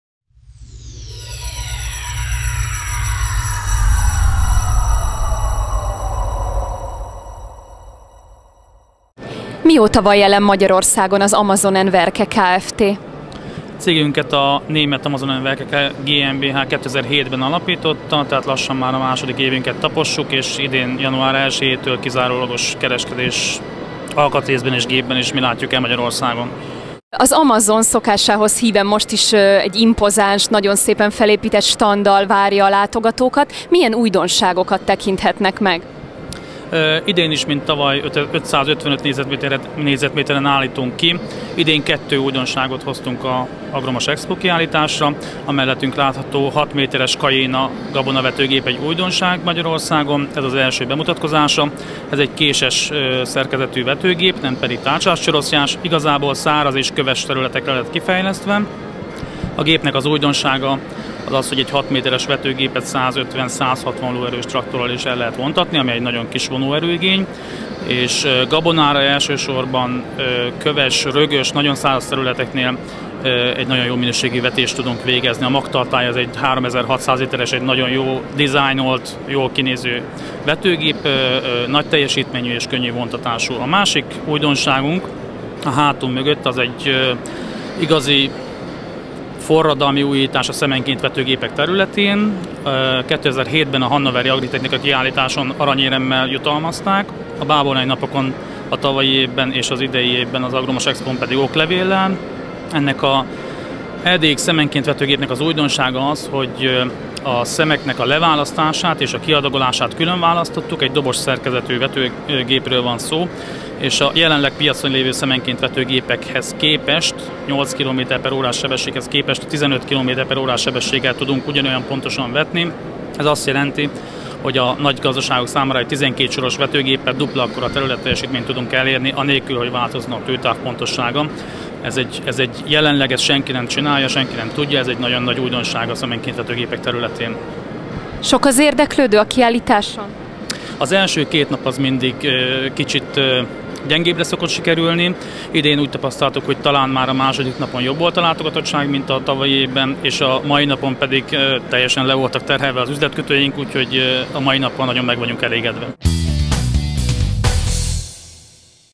Amazone munkagépek az Agro + Mashexpón
Interjú Kovács Tamás ügyvezetővel